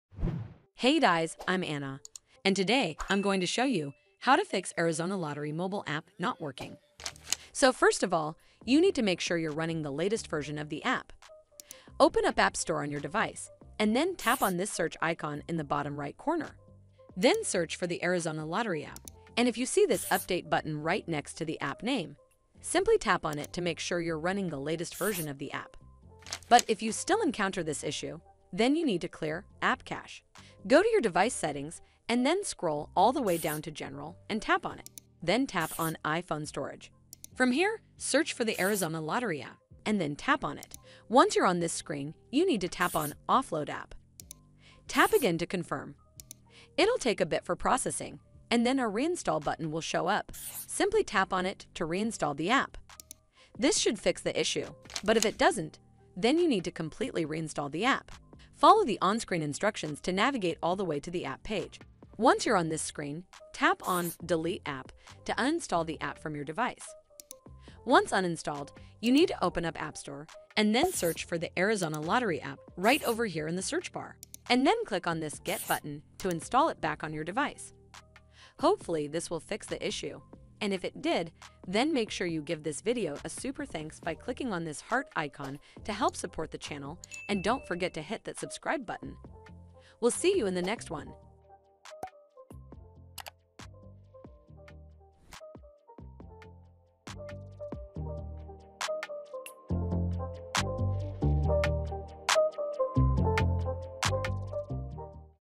AZ Lottery App Not Working: How to Fix AZ Lottery App Not Working
In this video, I'll show you How to Fix AZ Lottery App Not Working. This is the easiest and fastest way to Fix AZ Lottery App Not Working. Make sure you watch until the end of this video to find out How to Fix AZ Lottery App Not Working on ios. These methods work on iOS 11 as well as iOS 12, iOS 13, iOS 14, iOS 15 and iOS 16. Hope you enjoy!

Video Parts:
00:00 Intro: How to Fix AZ Lottery App Not Working
00:07 1.Solution: Update AZ Lottery App
00:27 2.Solution: Clear AZ Lottery App cache files
00:57 3.Solution: Delete and Reinstall AZ Lottery App
01:19 Outro: Ending